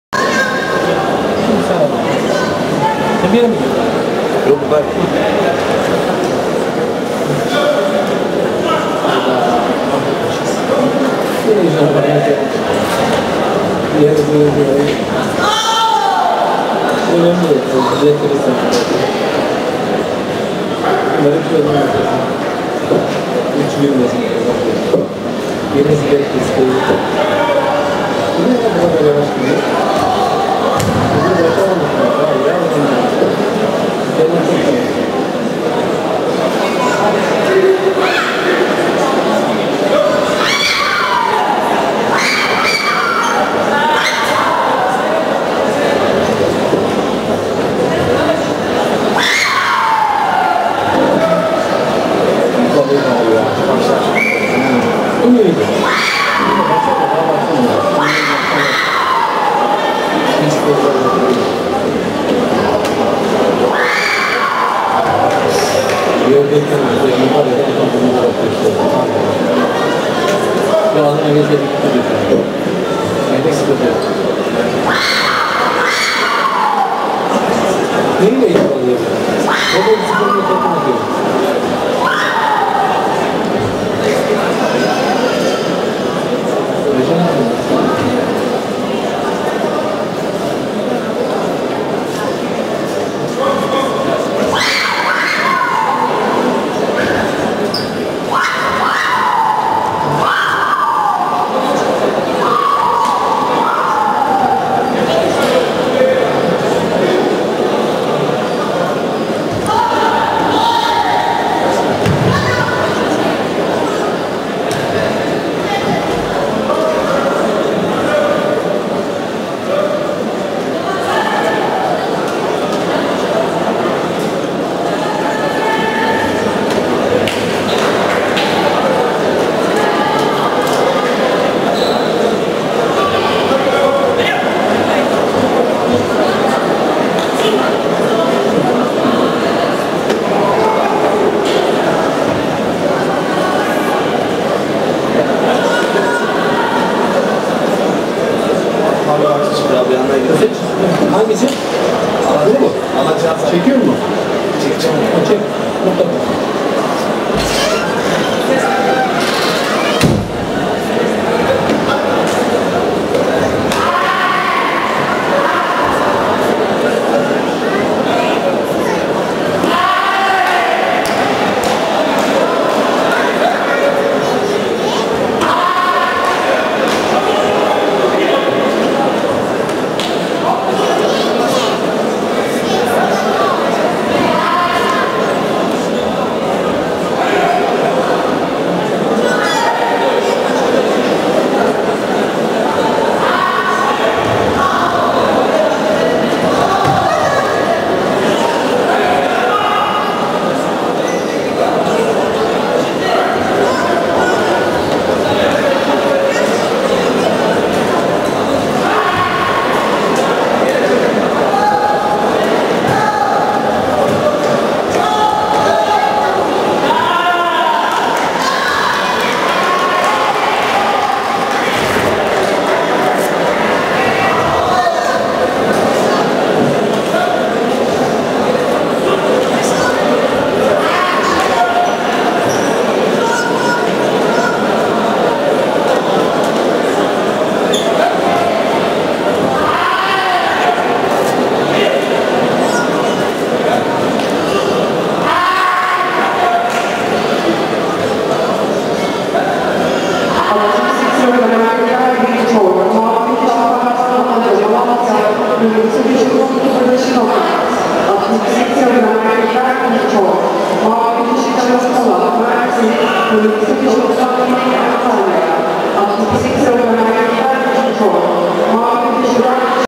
57kg Fatma Sezer - Rabia Tugce Isman (2012 Turkish Taekwondo Championships Under -21)
Turkish Taekwondo Championships Under 21.
14-29 May 2012 - Antalya -Turkey